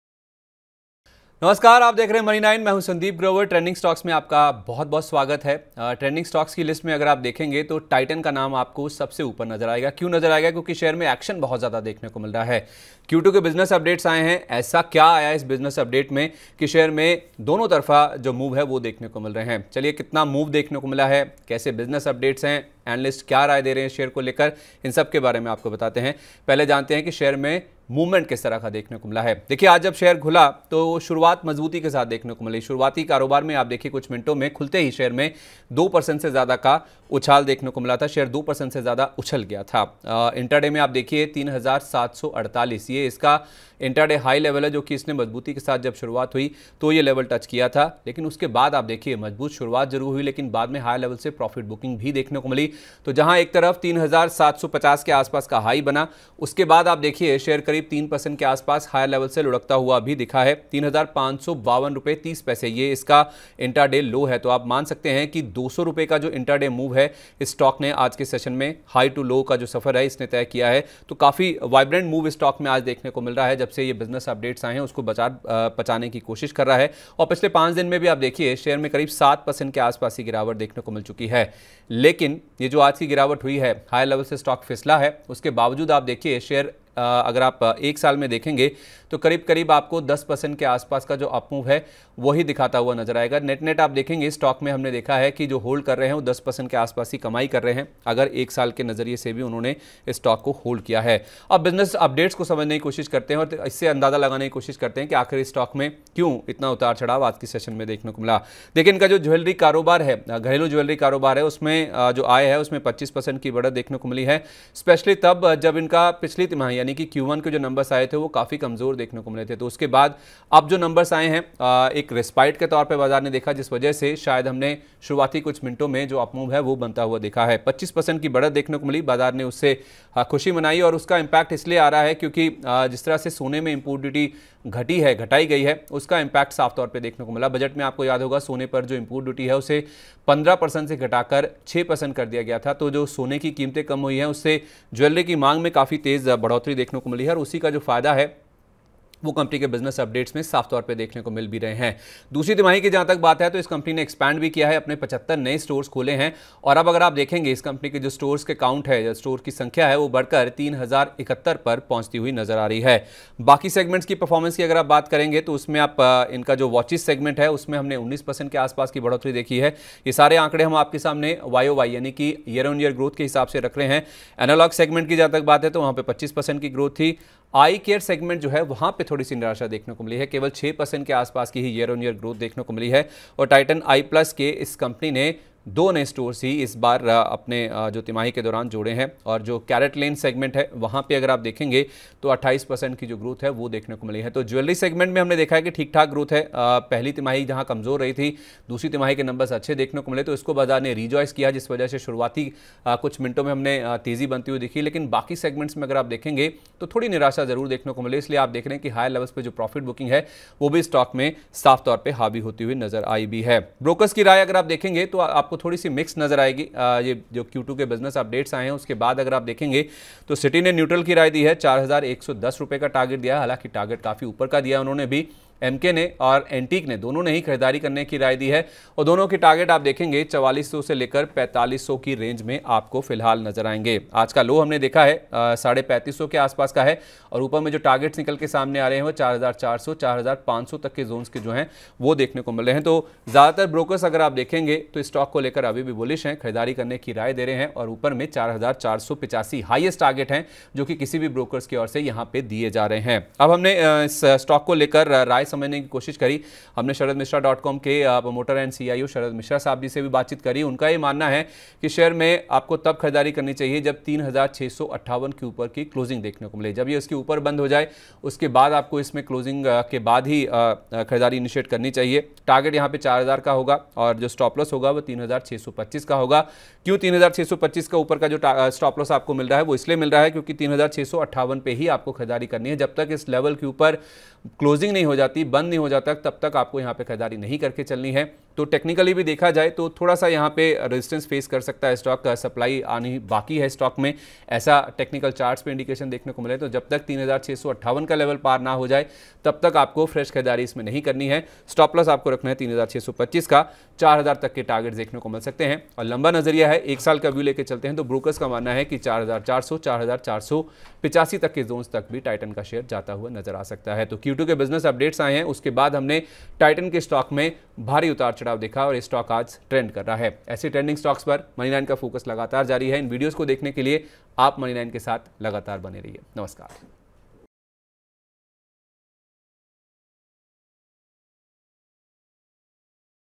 Titan Share Price : Q2 के बिजनेस अपडेट्स के बाद Titan Share में आए उतार-चढ़ाव में क्या करें?
Stocks To Buy : Titan Share में आज के उतार-चढ़ाव के पीछे क्या है बड़ी वजह? कैसे रहे Titan के दूसरी तिमाही के बिजनेस अपडेट्स? Q2 के बिजनेस अपडेट्स जारी होने के बाद Titan Share पर क्या है ब्रोकर्स की राय? आज के भारी उतार-चढ़ाव के बाद Titan Share में खरीदारी करें या बिकवाली? जानिए इस वीडियो में